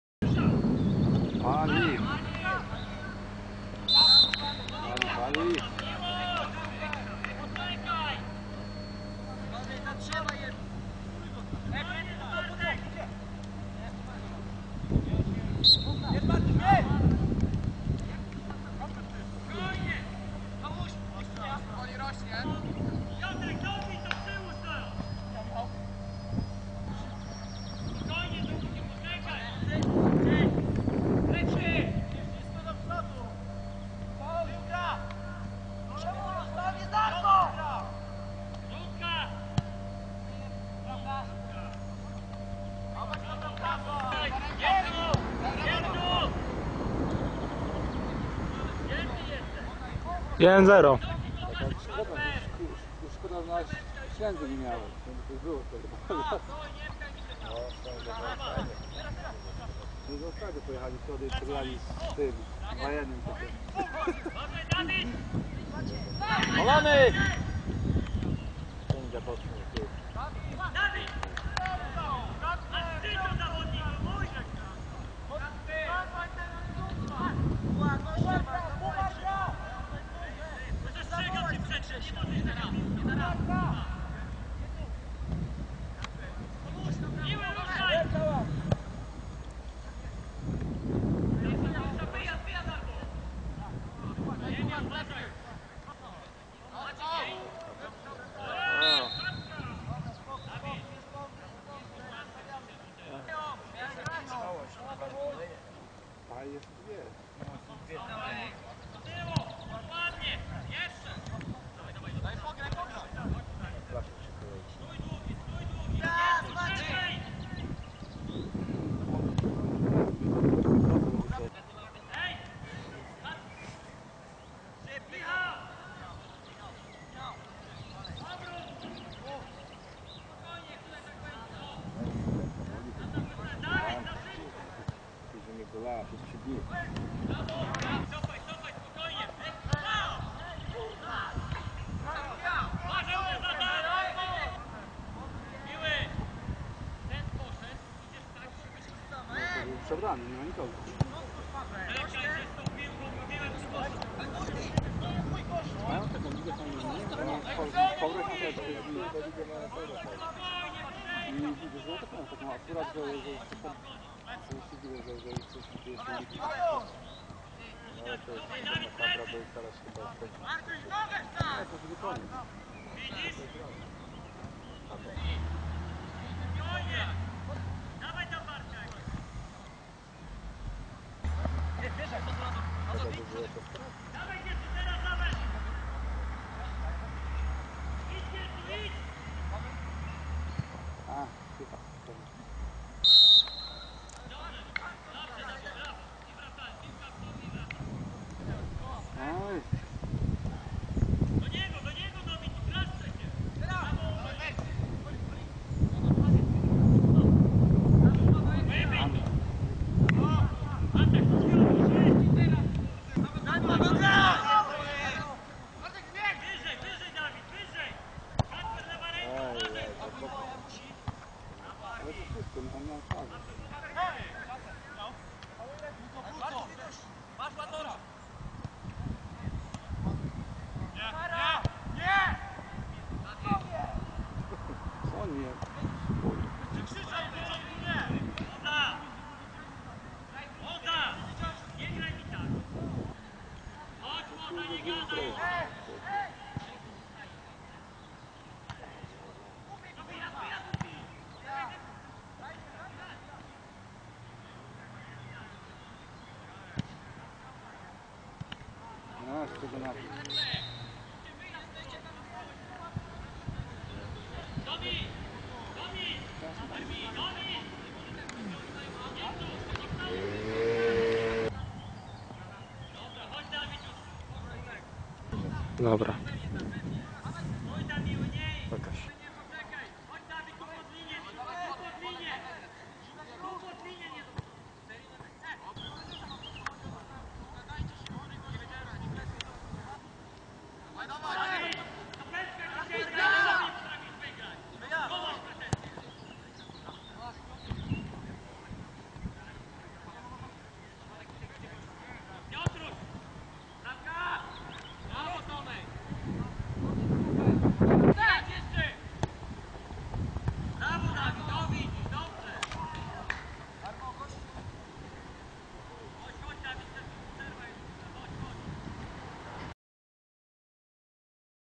Kultowe teksty trenerskie
Dobrze znane powiedzenia, wszystkim zawodnikom trenera Radka, z meczu okręgowej ligi juniorów.
Polecam oglądać z podpisami